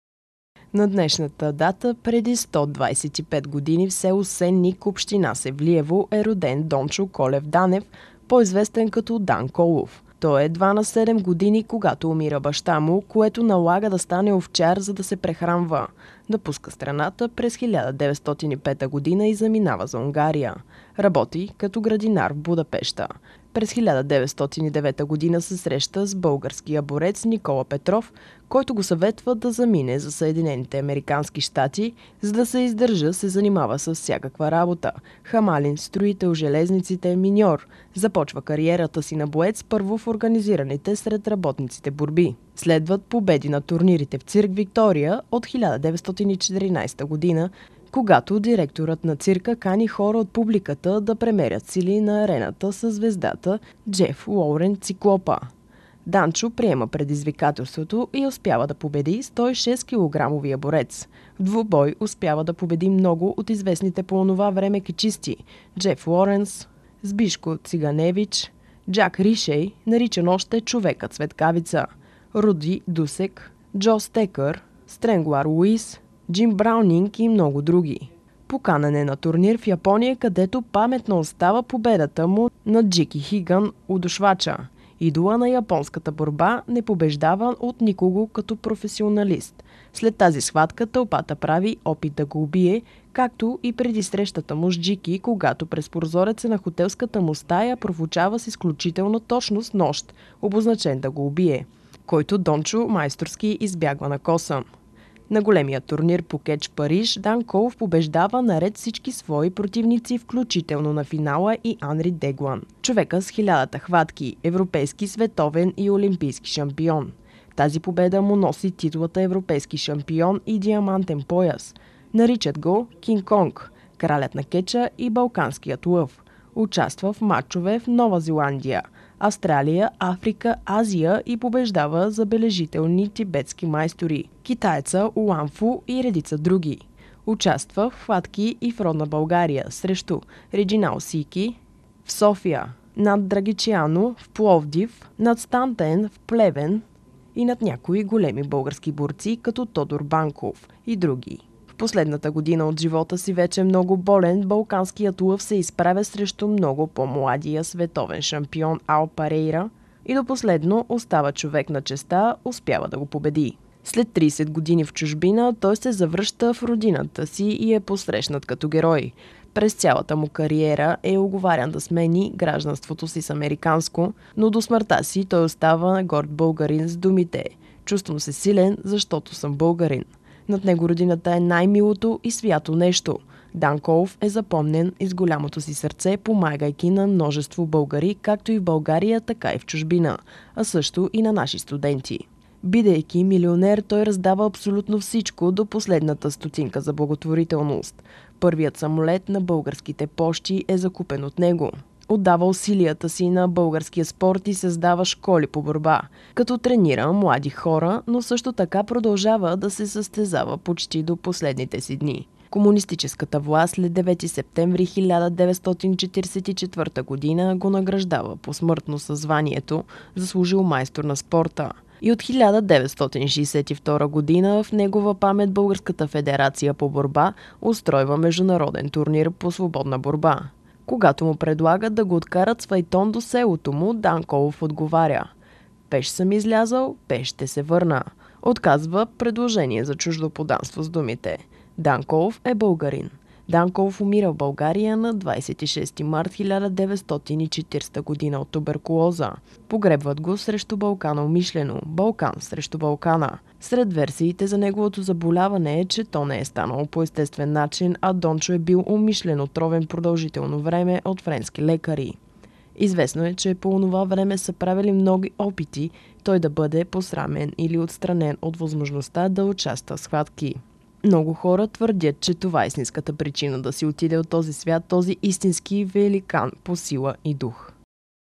Почит. 125 години от рождението на Дан Колов /27.12.2017 г./
Днес се навършват се 125 години от рождението на един от най-великите български спортисти за всички времена - легендата Дан Колов